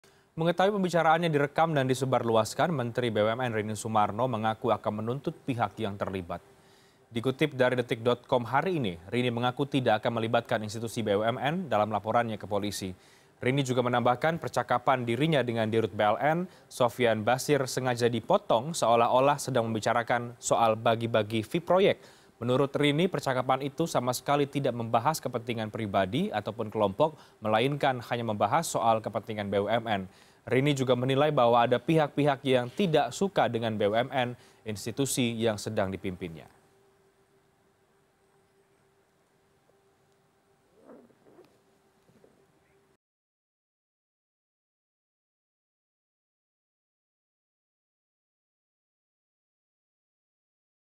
Rekaman Tersebar, Menteri Rini Lapor Polisi
Mengetahui pembicaraannya direkam dan disebar luaskan, Menteri BUMN Rini Soemarno mengaku akan menuntut pihak yang terlibat.

Like & Follow Facebook:
CNN Indonesia

cnnindonesia